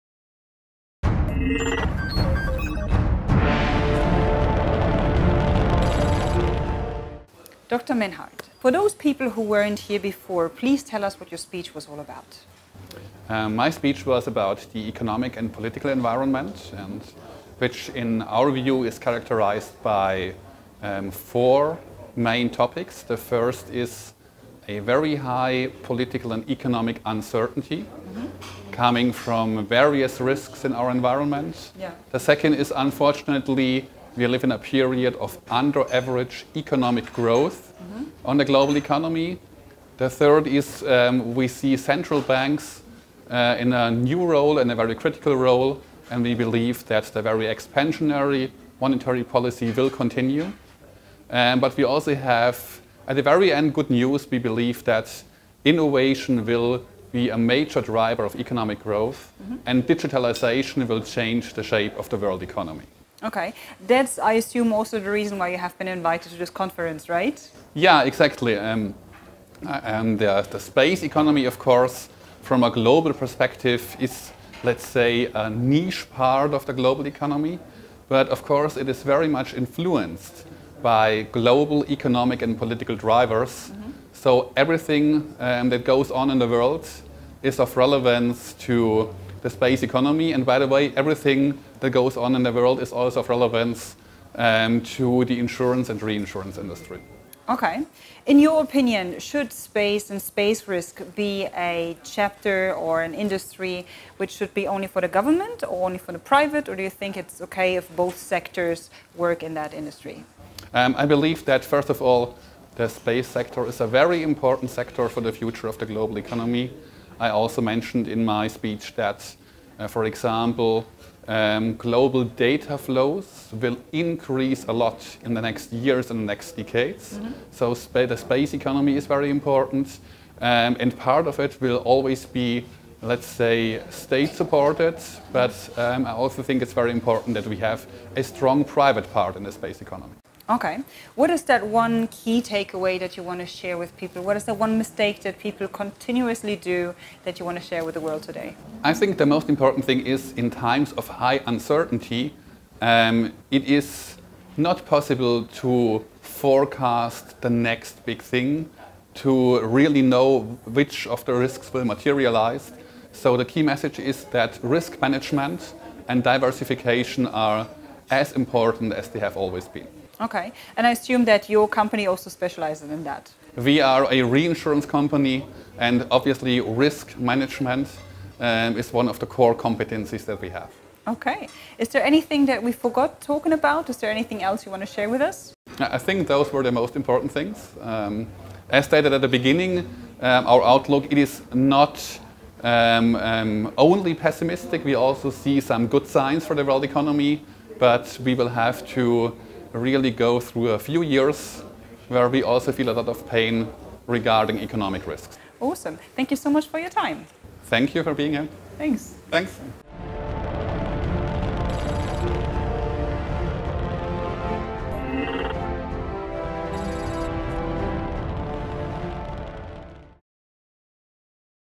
Dr. Michael Menhart, Chief Economist, Munich Re
WSRF 2016 Event Key Note Speaker Dr. Menhart of Munich Re gives an illuminating view of the current economic and political environment. The uncertainty, the risks and the challenges that we face.